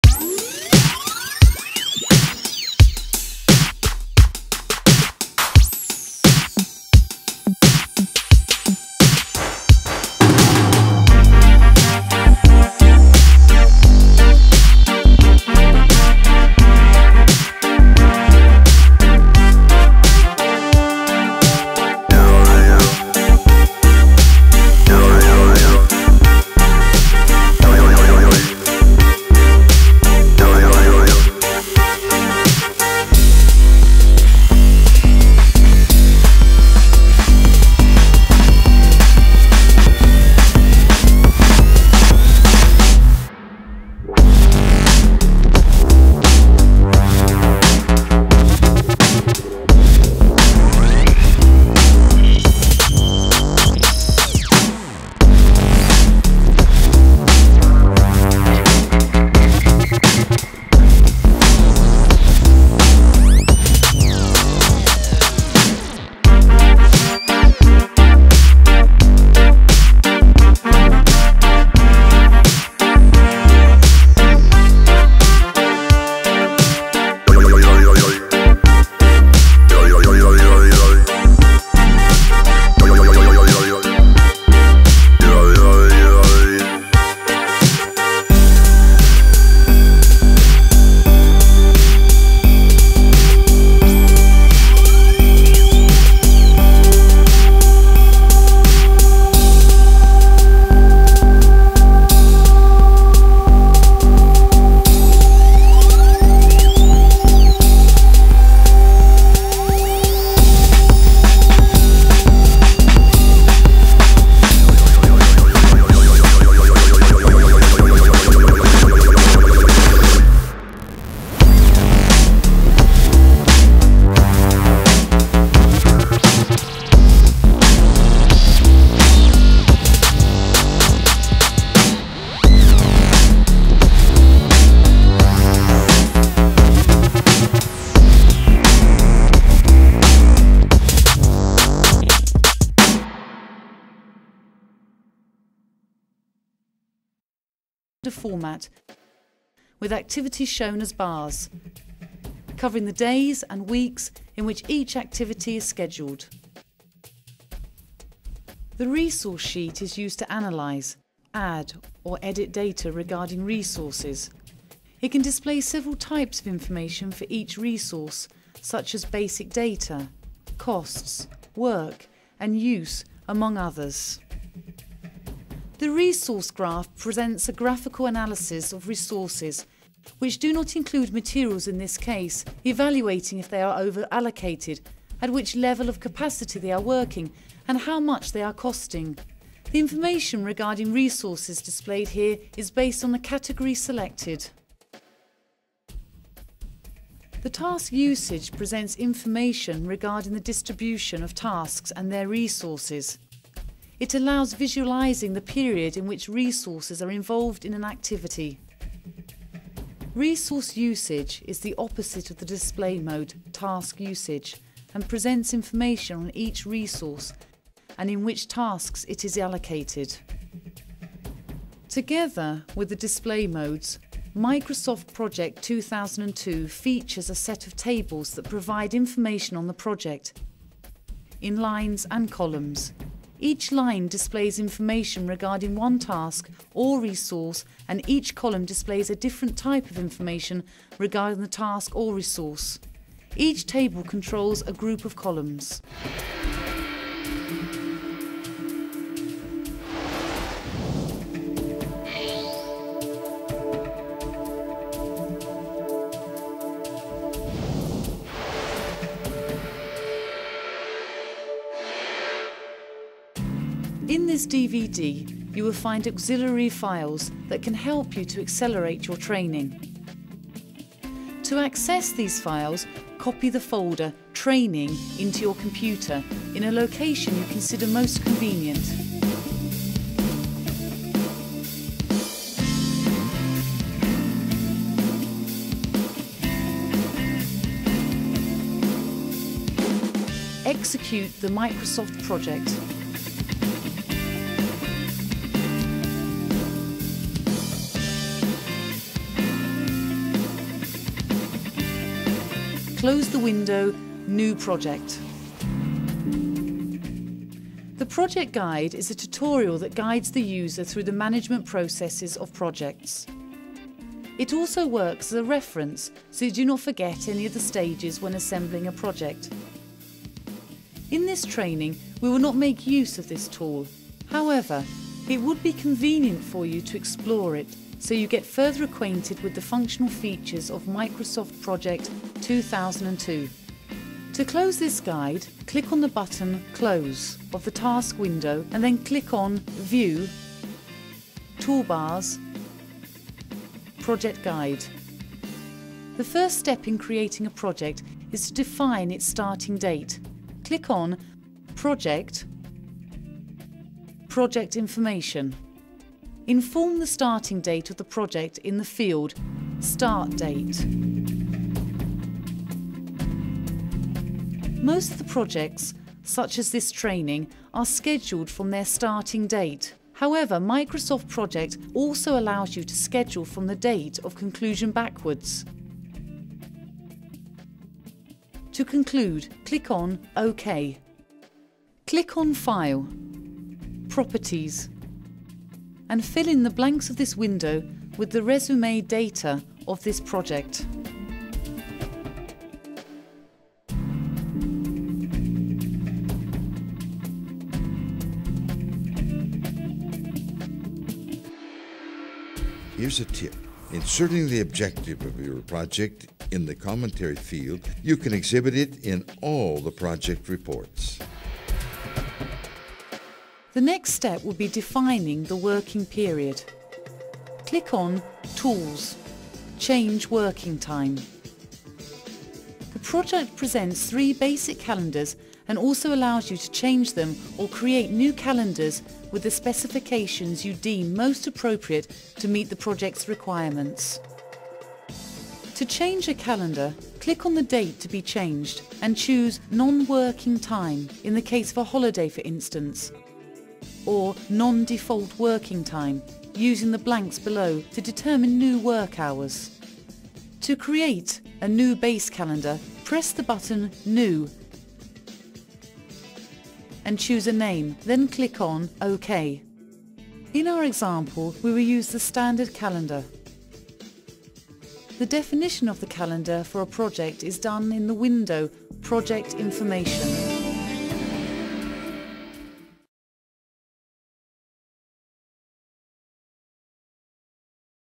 Part 2/8 - Microsoft Project Online Course
Part 2/8 of the Microsoft Project online course produced by Ricardo Vargas in 2003.

The course objective is to present, in a simple and interactive way, the basic concepts of project management, using one of the most popular tools in the world, the Microsoft Project.

Content of this Part:

- Project 2002 Presentation
- Planning with Project 2002

Also available in Brazilian Portuguese and Spanish at Youtube.

Ricardo publishes videos on project management, agile methods and transformation, including tutorials, courses, case studies, career advice, among others.